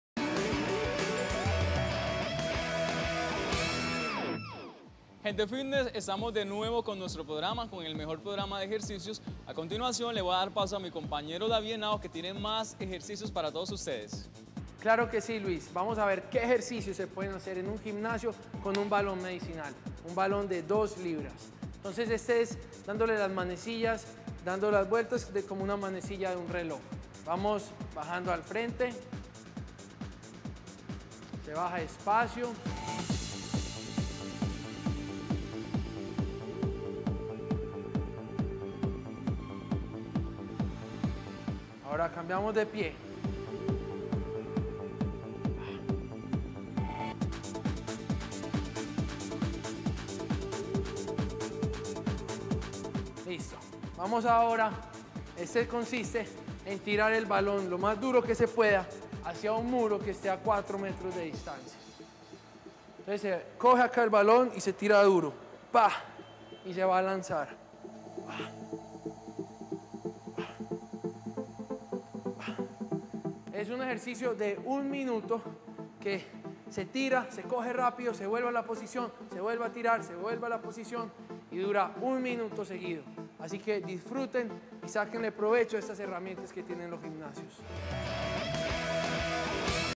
Ejercicios para tonificar cadera y eliminar celulitis. Rutina completa. Por David Henao.
Ejercicios para tonificar cadera y eliminar celulitis. Rutina completa